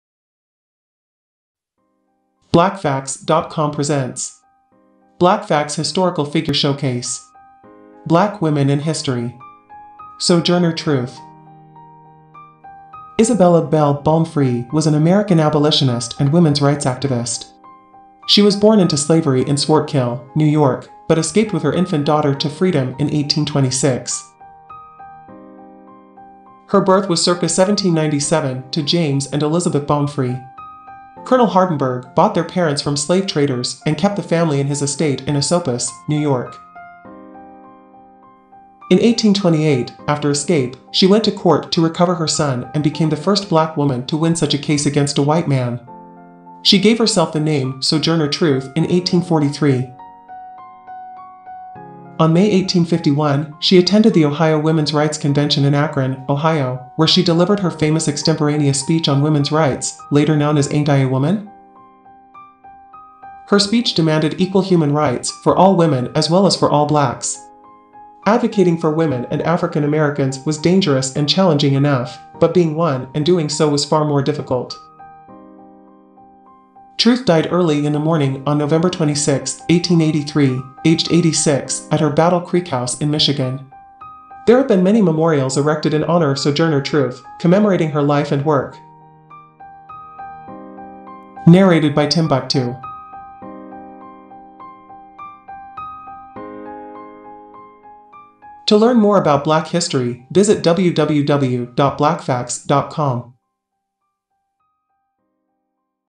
Sojourner Truth (Short)
BlackFacts.com Presents a Videos Series, Women in Black History, which delivers videos that showcase the life and accomplishments of inspiring women of color that you should know.

This Episode: SOJOURNER TRUTH